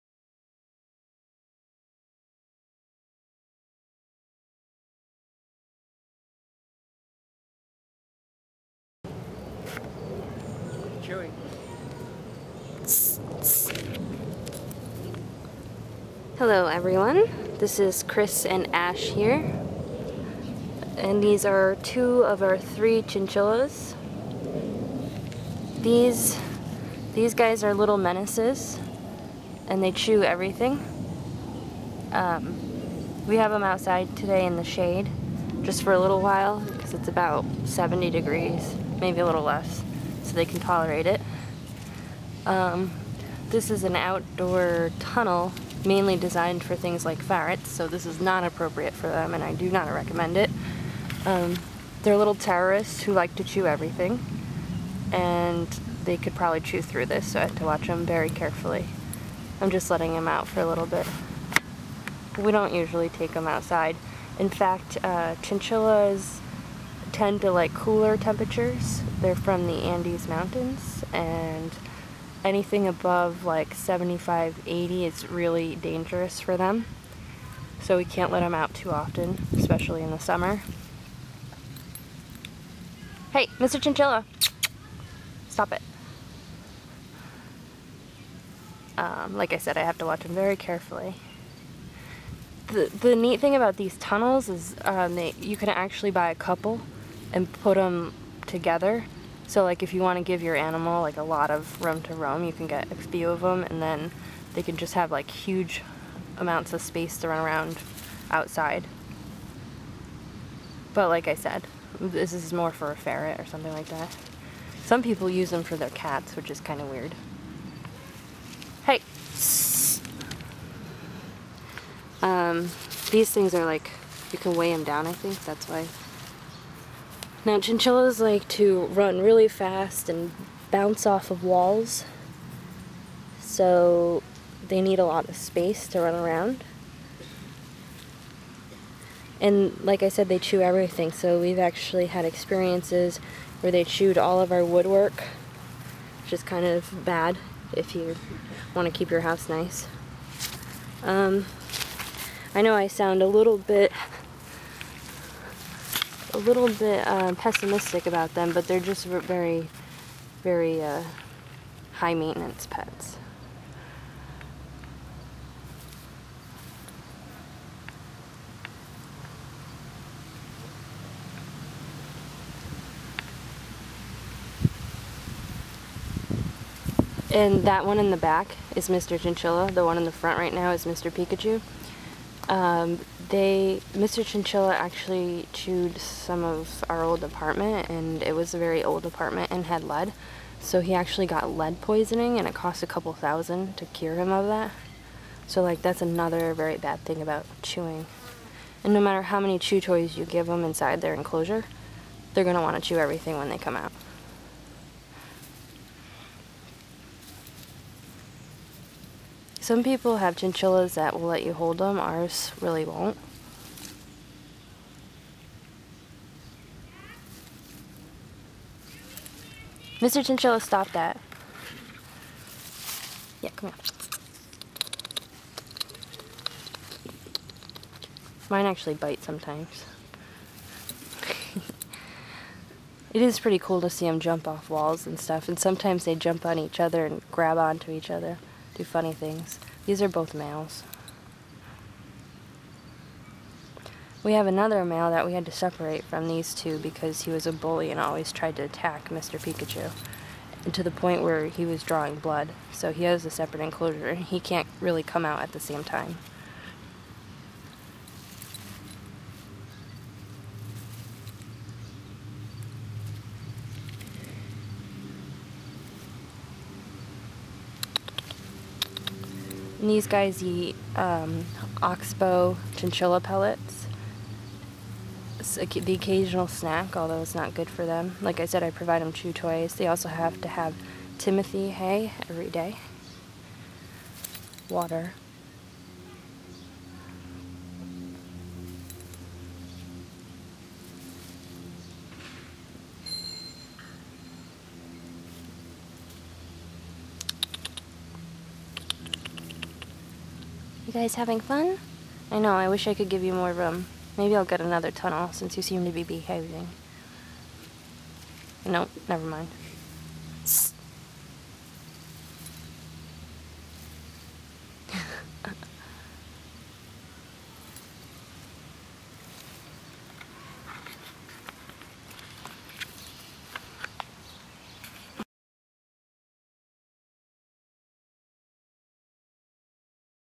Chinchillas Chinchillin' in the Backyard
We brought our chinchillas out for a little fresh air.  Be sure to check out our blog at chrisandash.wordpress.com and the Reptile-Parrots Forum at reptile-parrots.com